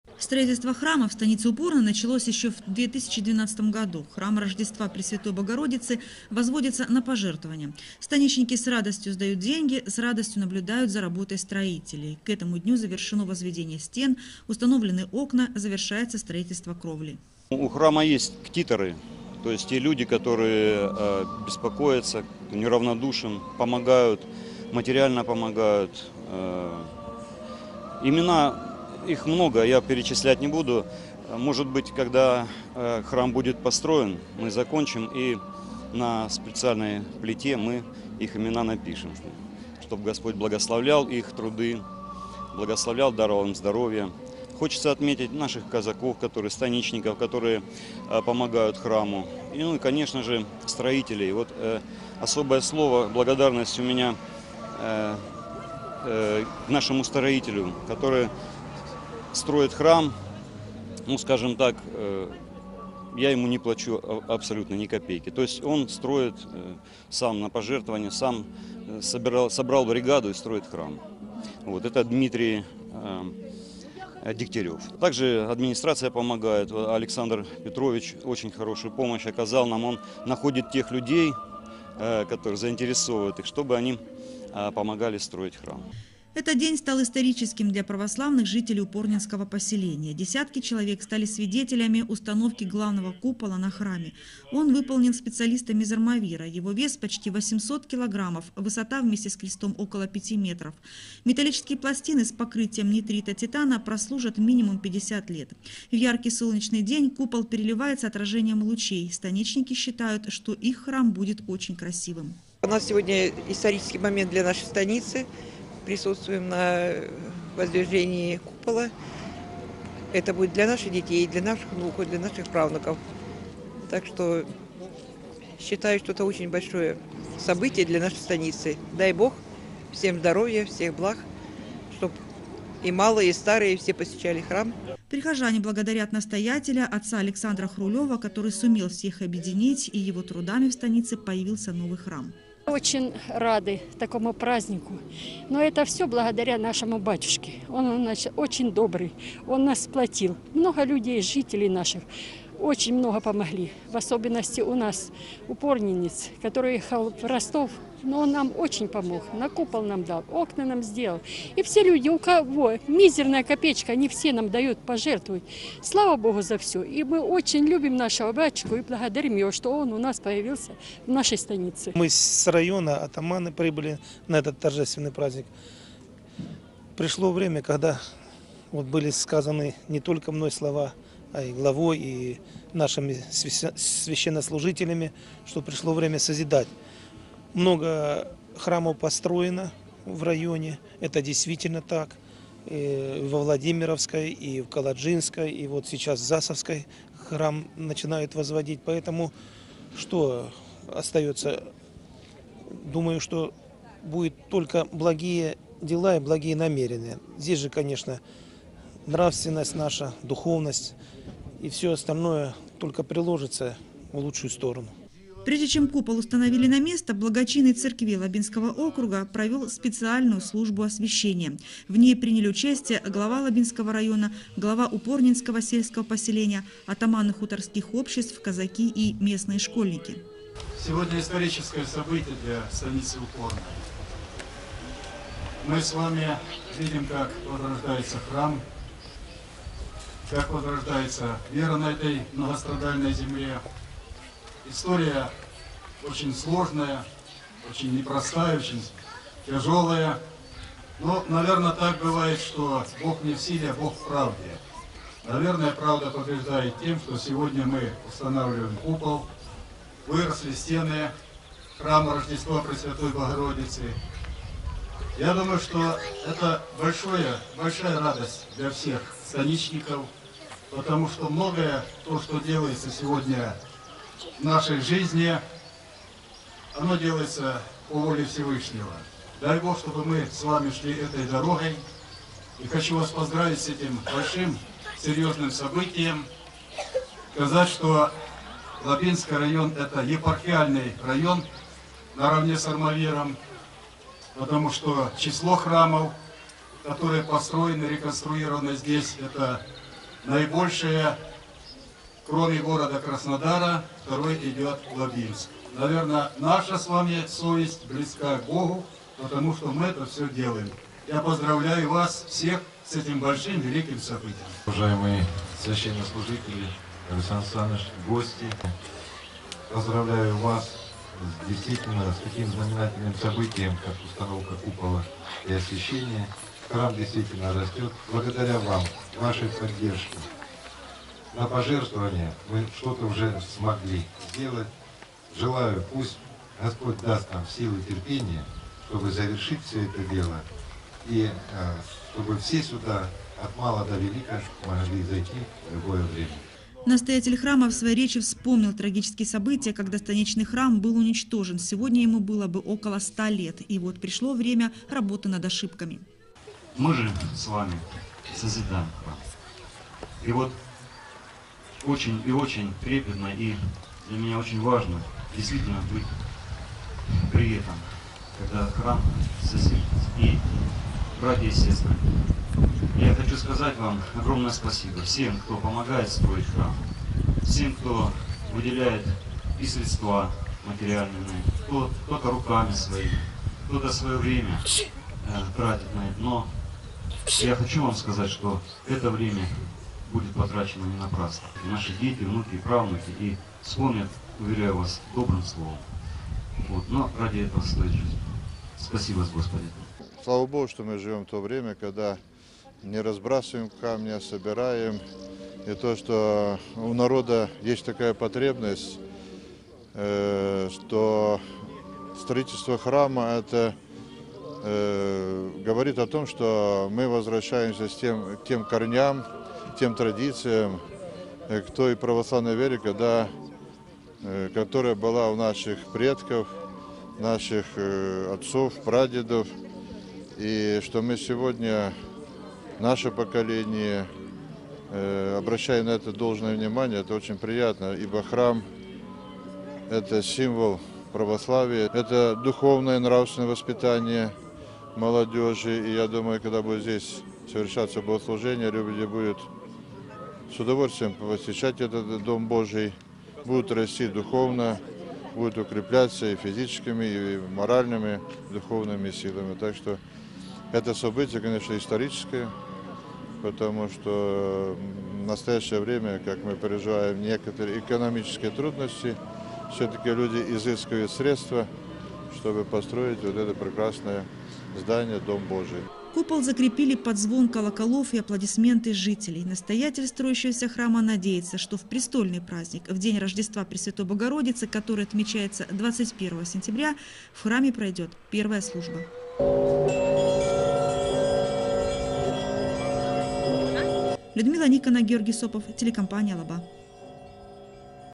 Установлен купол на строящийся храм в Упорной.
Установлен купол на строящийся храм в Упорной. Первое богослужение в храме Рождества Пресвятой Богородицы планируется провести уже этой осенью.